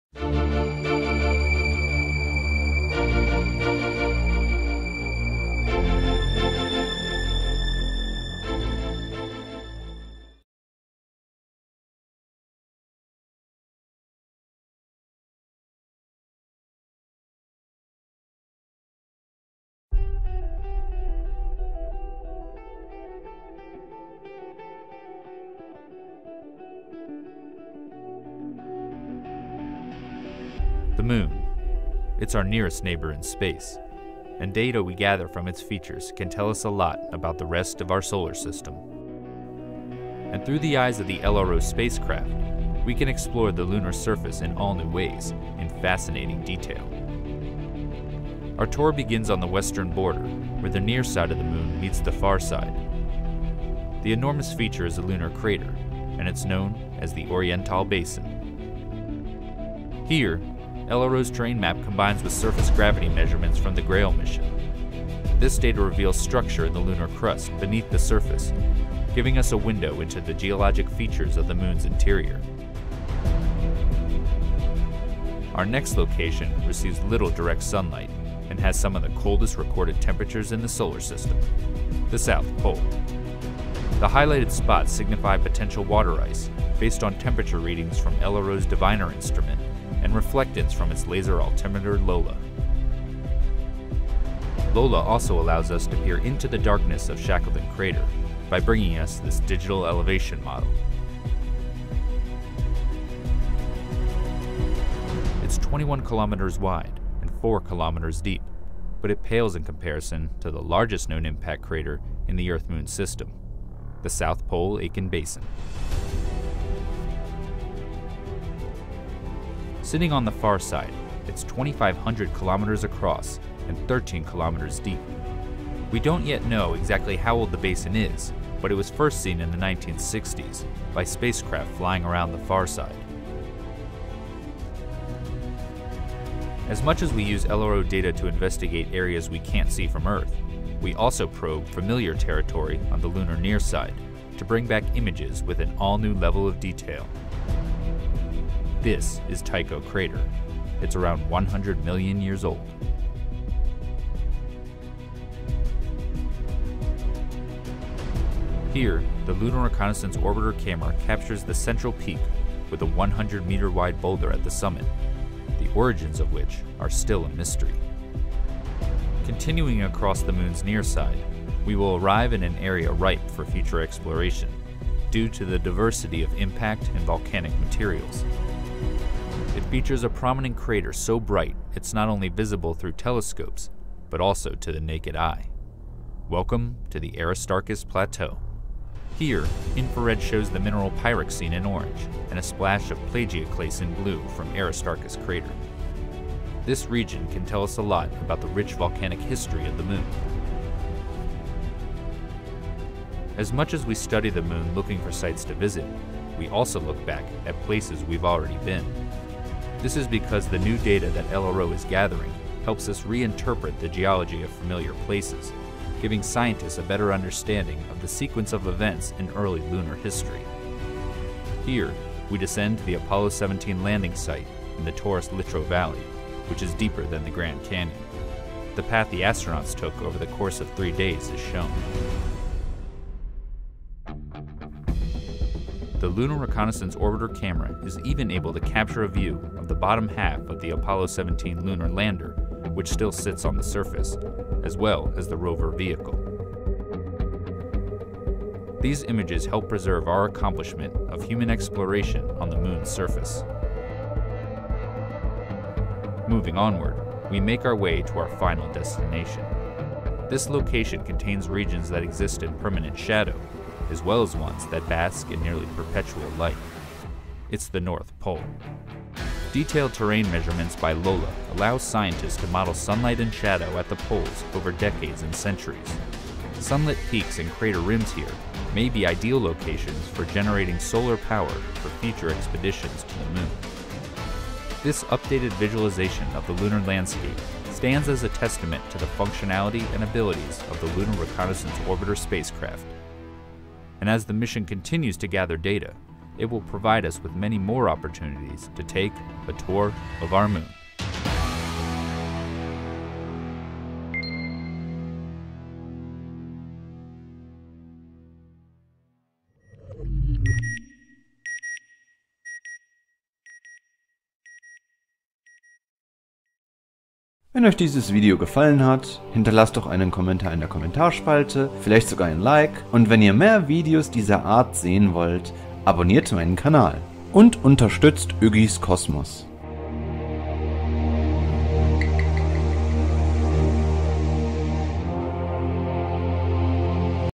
Mit Ultra HD zum Mond: Neue Erkenntnisse über den Erdtrabanten [Yggi's Kosmos]
Der Mond ist seit 4,5 Milliarden Jahren ein treuer Begleiter der Erde. NASA Goddard hat nun Dank der ultrahochauflösenden Kamera des Luna Reconaissance Orbiter UHD-Bilder des Mondes veröffentlicht.

Diese möchte ich euch, mitsamt den Untertiteln zu diesem Video, nicht vorenthalten!